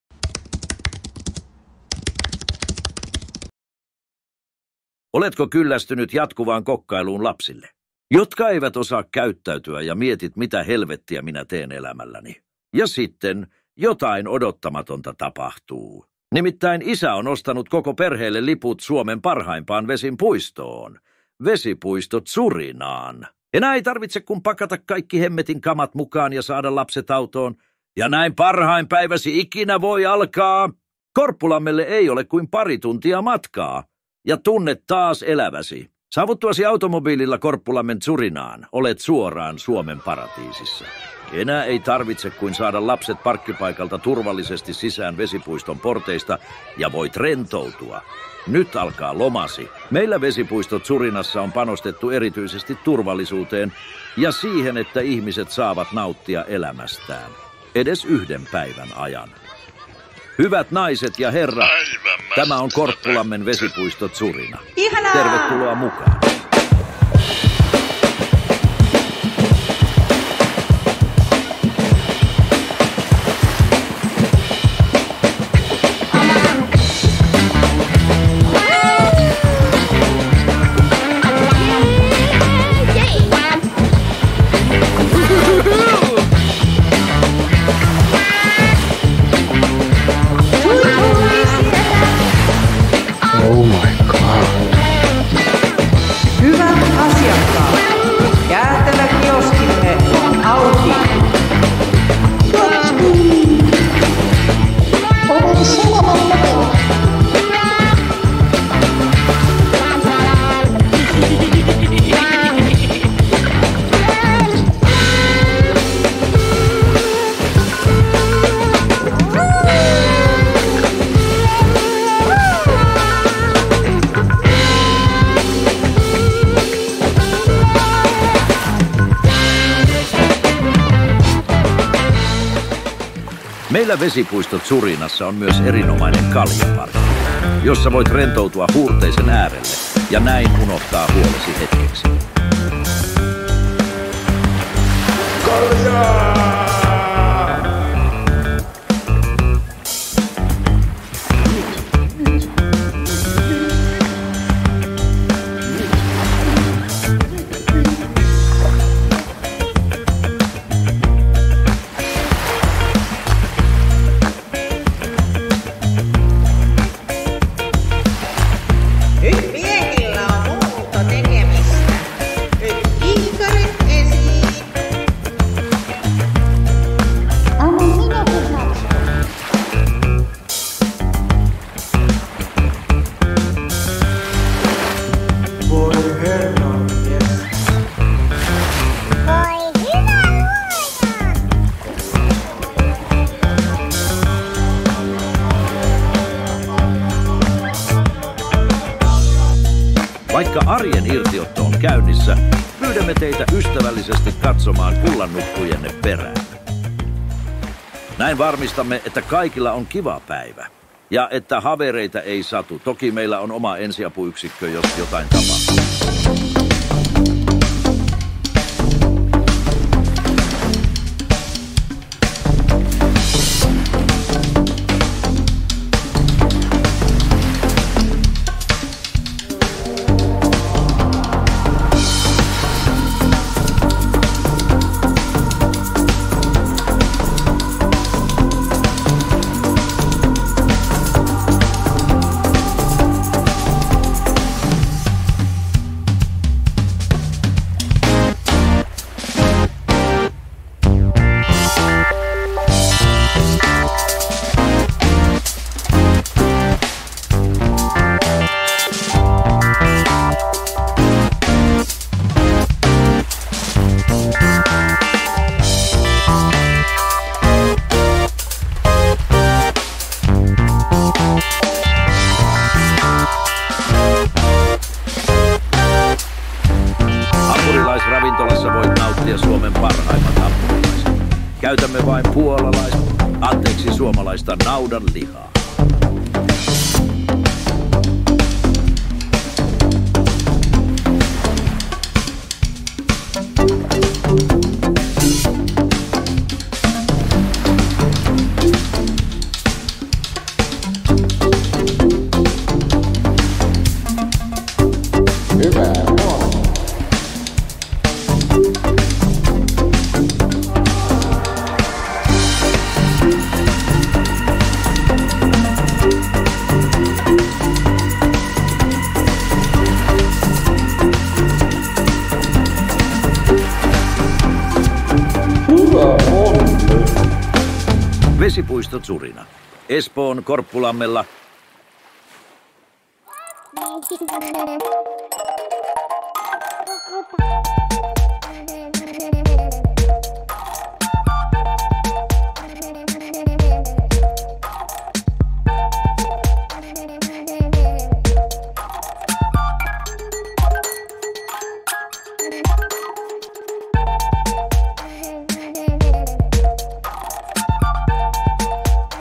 Vesipuisto Zurina [AI] - 1961
Vesipuisto Zurina toimi vuoteen 1961 asti kunnes jostain syystä se lopetettiin.